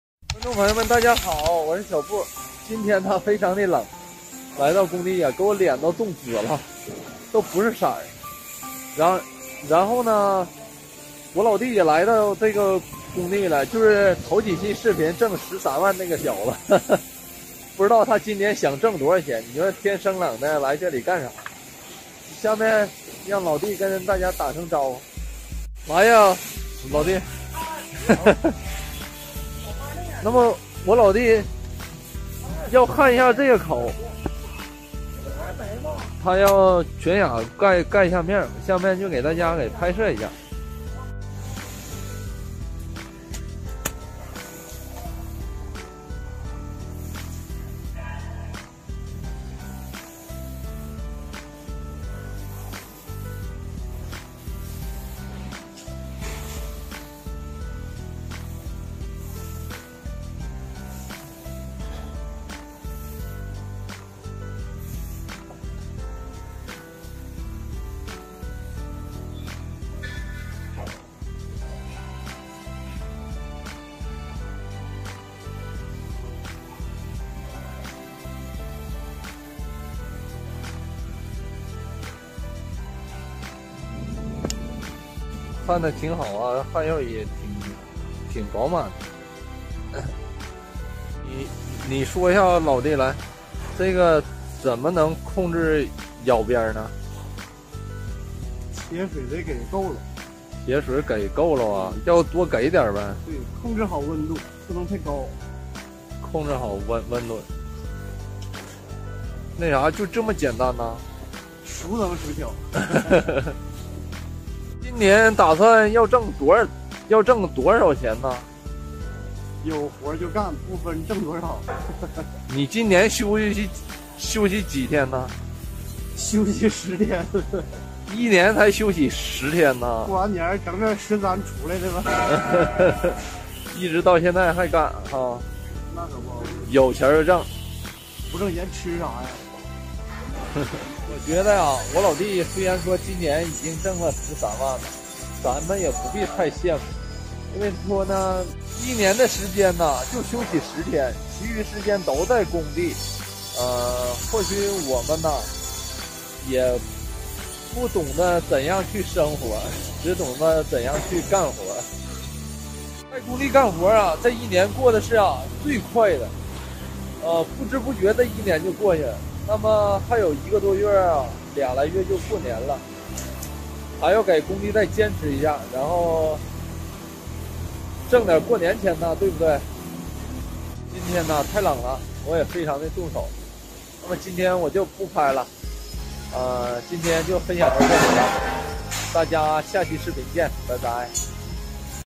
电焊工小伙今年挣了13万！他焊的怎么样，网友：不懂生活只知干活
生活,自制,VLOG,学习,工资,记录